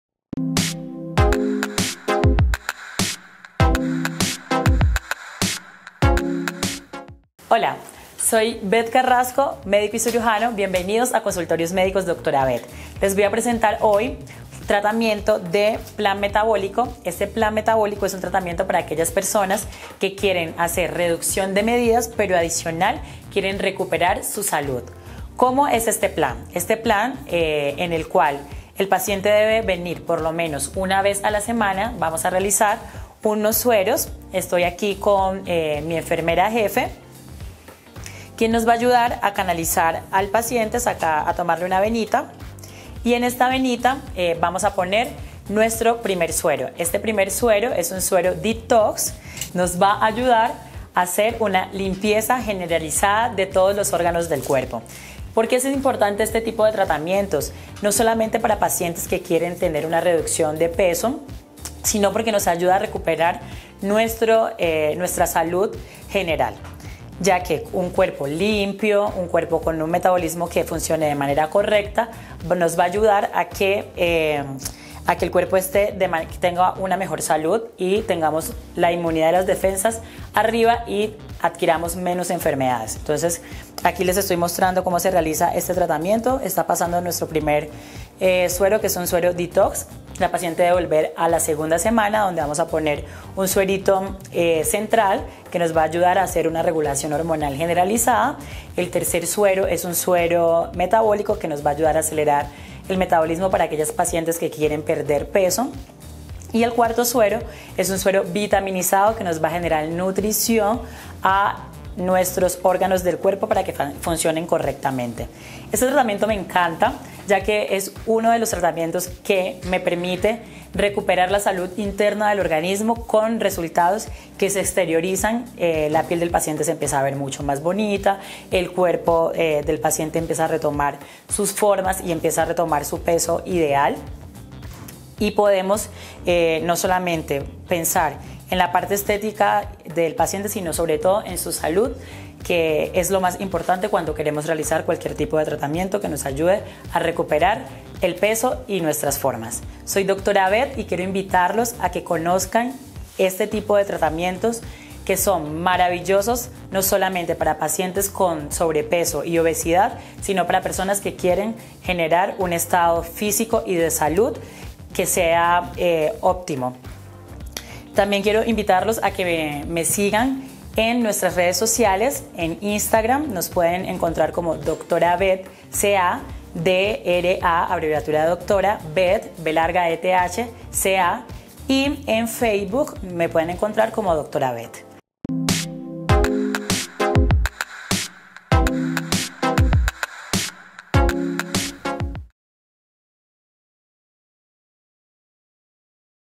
Plan metabólico y suero - HogarTv producido por Juan Gonzalo Angel
Las nuevas terapias con sueros o sueroterapias son la alternativa de moda que ayuda en temas de salud y estética. Es una opción terapéutica que desintoxica el cuerpo y ayuda a su autocuración fortaleciendo su sistema inmunológico a la vez que ayuda en la eliminación de toxinas.

Contacto Hogar TV